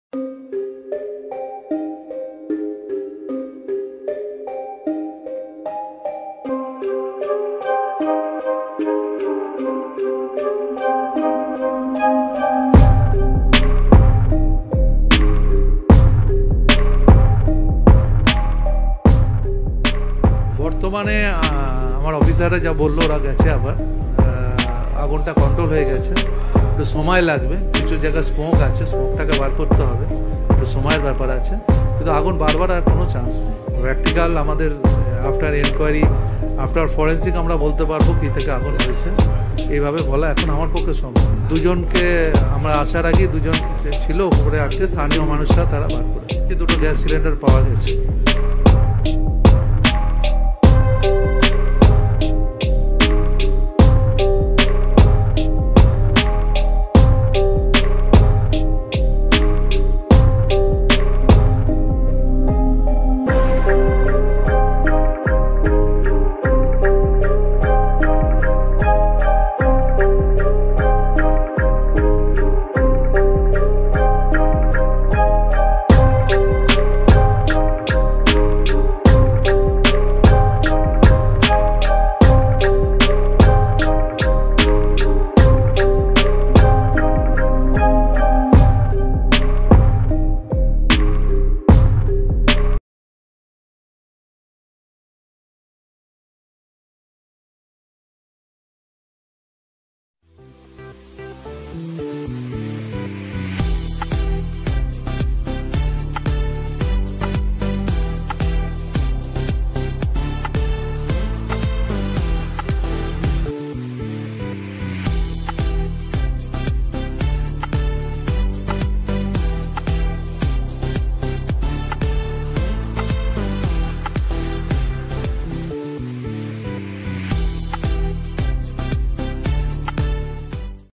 Kolkata ke Tiratti Bazar areas ke Sun House ke 4th floor mey Aag, Fire Minister bhi Bhi Aag ka jaeza
कोलकाता के तीरट्टी बाजार इलाके में सन हाउस की चौथी मंजिल पर आग लग गई। चार फायर गरज और भीड़ मौके पर तो दो आग लगी।